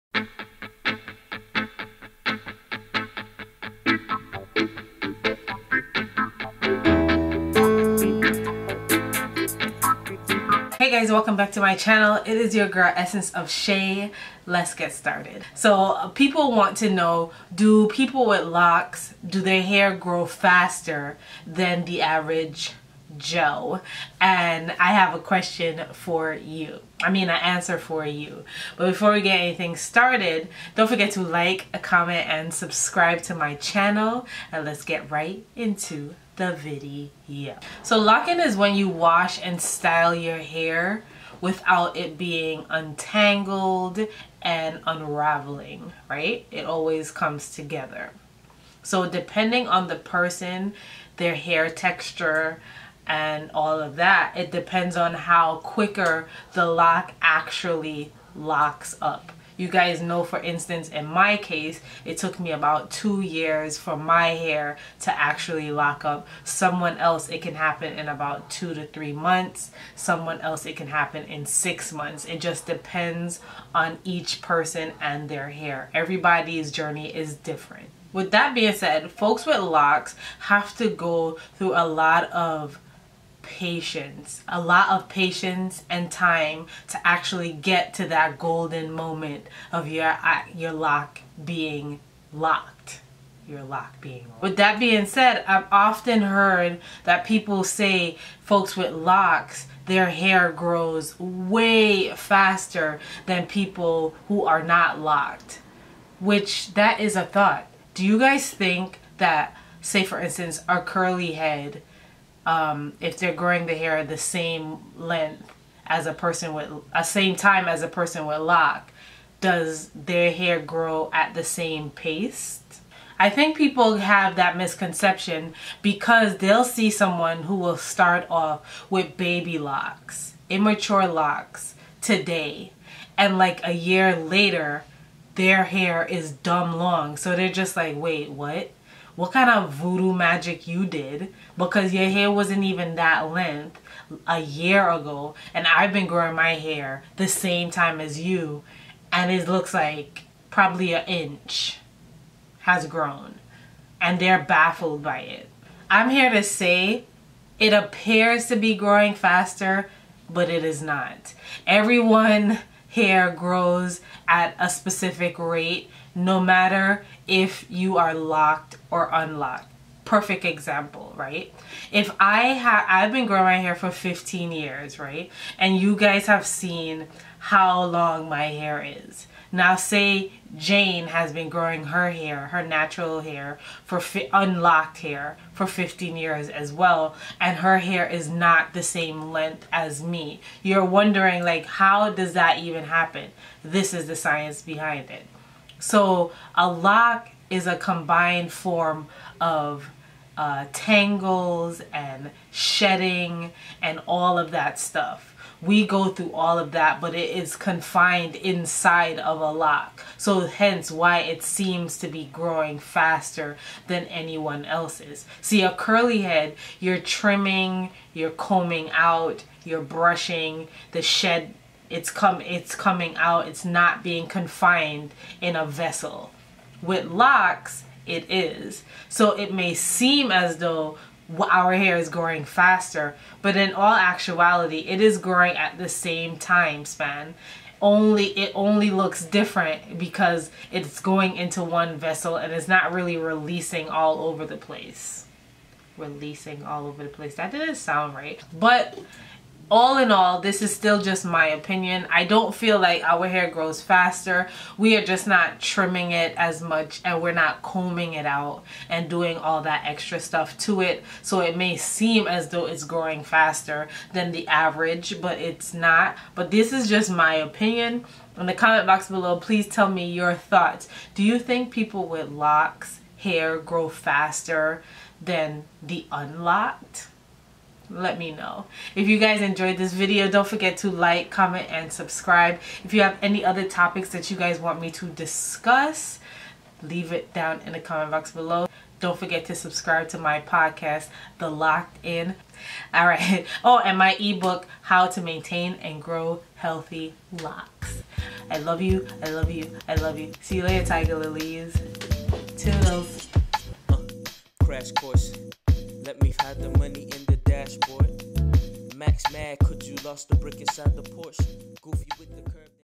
WHY DO LOCS/DREADLOCKS GROW SO FAST & LONG I ESSENCEOFSHAY
HEY TIGERLILEEEE'S! WATCH ON HD!!! Why do locs/dreadlocsks gro so fast and long? This is a questions lot of folks ask, I have a couple reasons why! Keep on watching!!

~Want to send me stuff? Send it to my P.O.Box:
P.O.BOX 1001